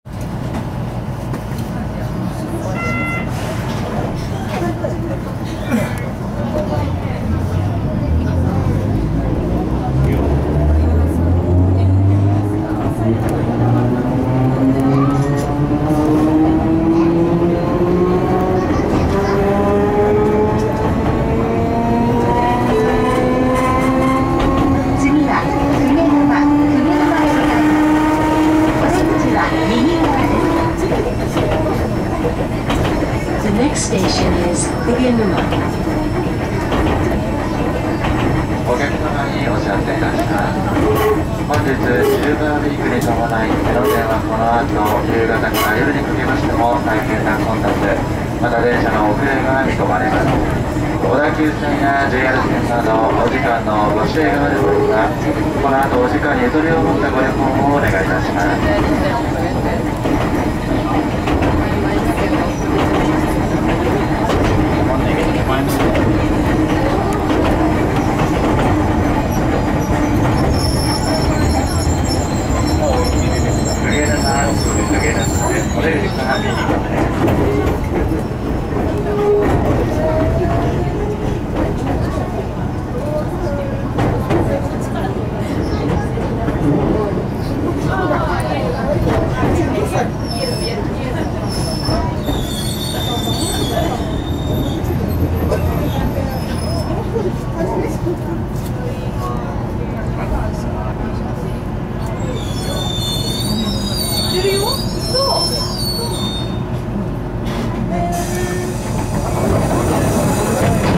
江ノ電 柳小路〜鵠沼駅 Enoden Yanagikoji to Kugenuma
江ノ電 柳小路〜鵠沼駅までの路線です。江ノ電って単線なんですね。今日まで知りませんでした。

Enoden willow alley - It is the route of Kugenuma Station. It's single line that I Enoden. I did not know until today.